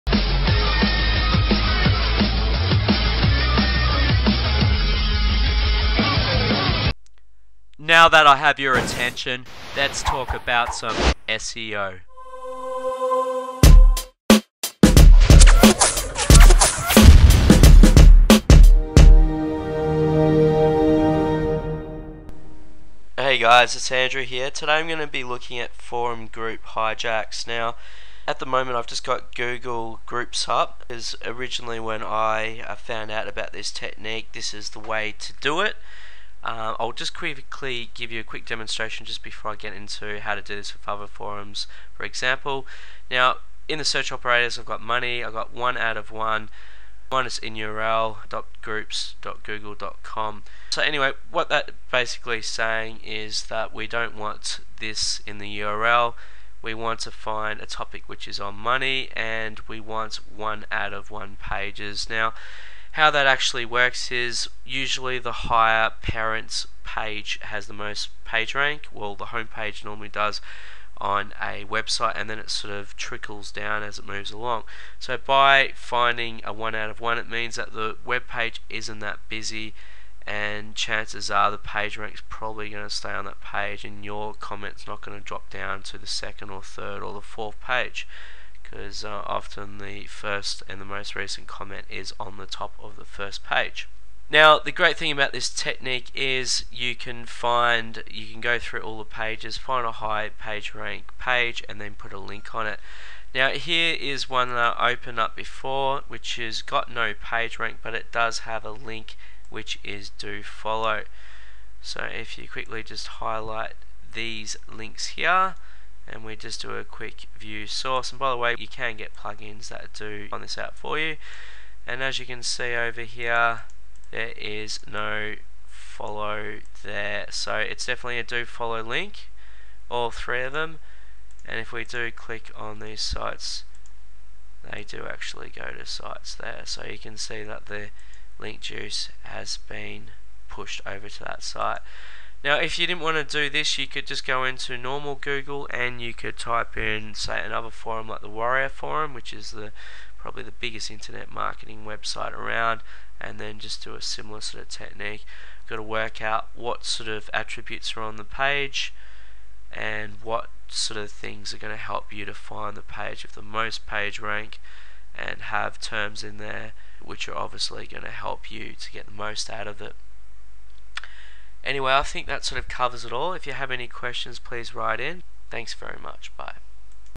Online Marketing - Tutorial 31 - Forum Group Hijacks
I talk about Forum Group Hijacks and techniques relating to parent pages, page rank, commenting, search operators, do follow links, no follow links, warrior forum technqiues and link juice. Find out more about our services

Other profiles
Andrew Kelly Facebook
Andrew Kelly's Book